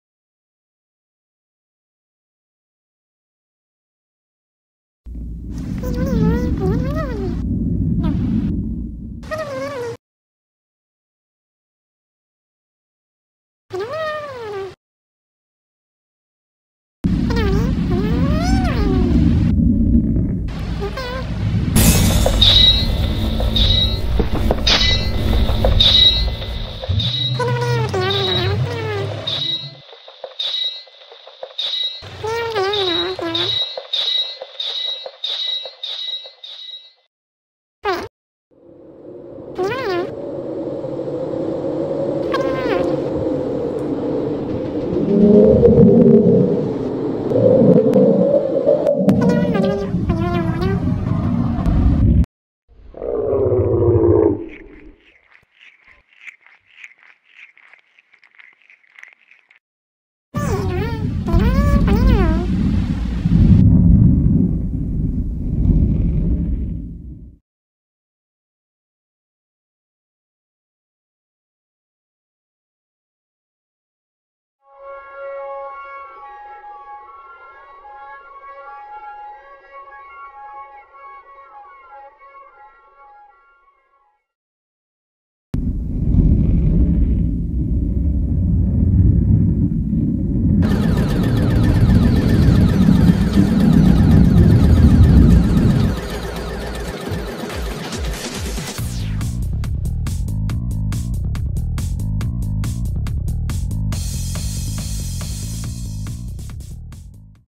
Mash-up project from YiChun Wang
Illustration Animation Student YiChun Wang's Mash-up project from Kingston University Level 5. It combines elements from both the 3D workshop and the color workshop. This is my first attempt at creating a 3D animation, and I also conducted numerous color tests towards the end of the video.